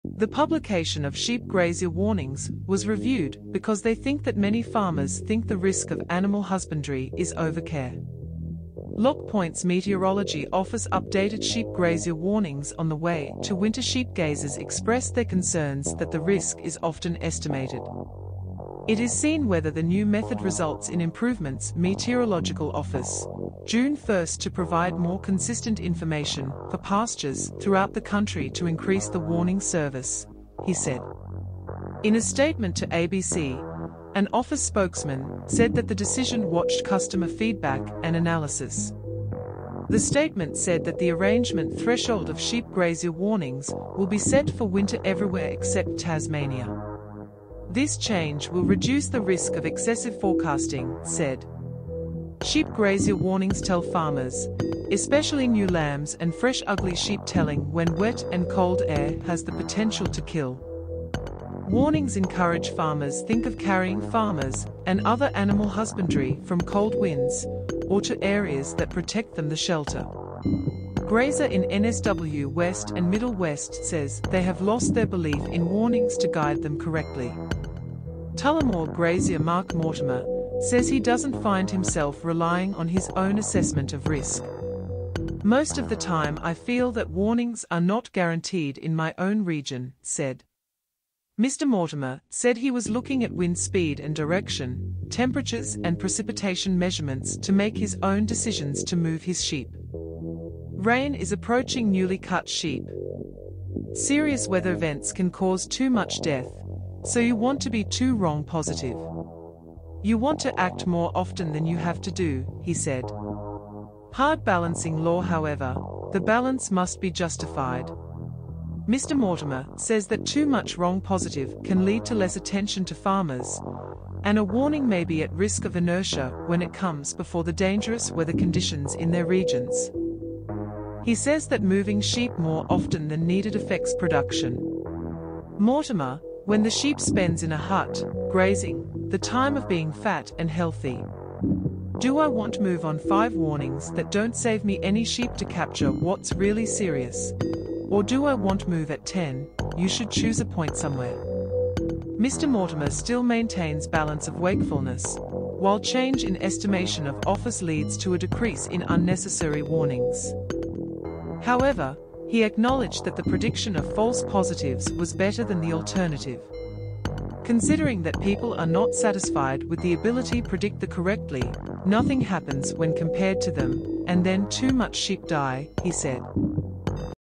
Bureau of Meteorology updates its sheep grazier warning service hoping it leads to less disruption
The publication of sheep grazier warnings was reviewed because they think that many farmers think the risk of animal husbandry is "overcare". Lock Points Meteorology Office Updated Sheep Grazier Warnings on the way to winter  Sheep Gazers expressed their concerns that the risk is often estimated. It is seen whether the new method results in improvements Meteorological Office, June 1 "to provide more consistent information for pastures throughout the country" to increase the warning service, he said. In a statement to ABC, an office spokesman said that the decision watched "customer feedback and analysis". The statement said that the arrangement threshold of sheep grazier warnings will be set for winter everywhere except Tasmania. "This change will reduce the risk of excessive forecasting."Said. Sheep Grazier warnings tell farmers, especially new lambs and fresh ugly sheep - telling when wet and cold air has the potential to kill. Warnings encourage farmers think of carrying farmers and other animal husbandry from cold winds or to areas that protect them the shelter. Grazer in NSW West and Middle West says they have lost their belief in warnings to guide them correctly. Tullamore Grazier Mark Mortimer says he doesn't find himself relying on his own assessment of risk. "Most of the time I feel that [warnings] are not guaranteed in my own region."Said. Mr. Mortimer said he was looking at wind speed and direction, temperatures and precipitation measurements to make his own decisions to move his sheep. Rain is approaching newly cut sheep. "Serious weather events can cause too much death, so you want to be too wrong positive. You want to act more often than you have to do," he said. 'Hard Balancing Law' However, the balance must be justified. Mr. Mortimer says that too much wrong positive can lead to less attention to farmers, and a warning may be at risk of inertia when it comes before the dangerous weather conditions in their regions. He says that moving sheep more often than needed affects production. Mortimer, when the sheep spends in a hut, grazing, the time of being "fat and healthy". “Do I want move on five warnings that don't save me any sheep to capture what's really serious?Or do I want move at 10?You should choose a point somewhere .. Mr. Mortimer still maintains balance of wakefulness, while change in estimation of office leads to a decrease in unnecessary warnings. However, he acknowledged that the prediction of false positives was better than the alternative. "Considering that people are not satisfied with the ability predict the correctly, nothing happens when compared to them, and then too much sheep die," he said.